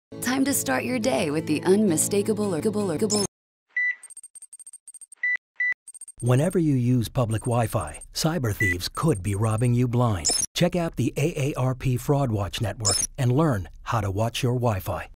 Fraud Watch Network: Watch Your Wi-Fi
Hackers are searching for ways to hack in to your Wi-Fi. Protect yourself.